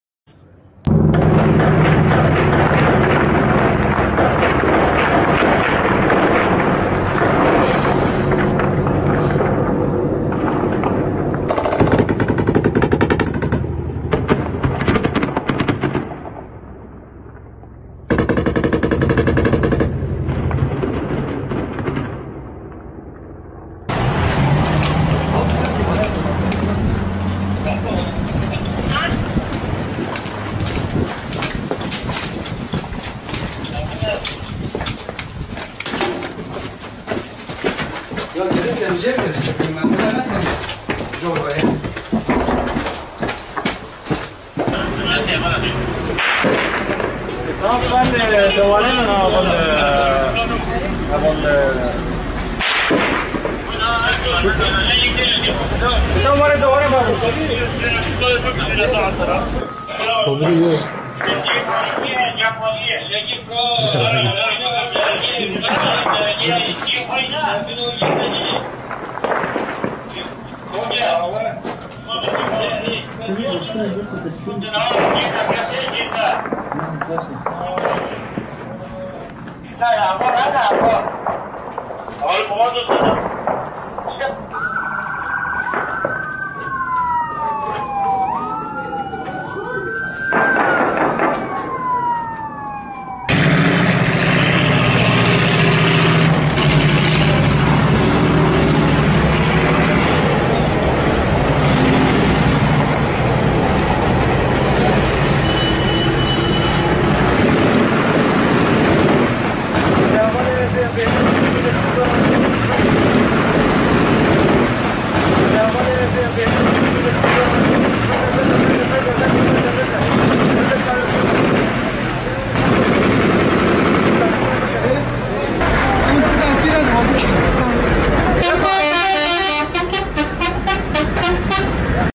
ISIS atacaram uma prisão na Síria entra confronto curdos da FDS (Forças Democráticas Sírias)
Mais imagens dos confrontos que ainda persistem entre radicais do Estado Islâmico contra os curdos da FDS (Forças Democráticas Sírias) depois que extremistas do ISIS atacaram uma prisão na Síria comandada pelos curdos para libertar vários combatentes do Estado Islâmico.
O grupo Estado Islâmico atacou há 6 dias uma prisão em Haka na Síria onde libertaram vários presos do grupo extremista. O ataque se transformou em insurgência e os curdos do FDS (Forças Democráticas Sírias) que controlam a prisão, estão em confronto contra os extremistas na Síria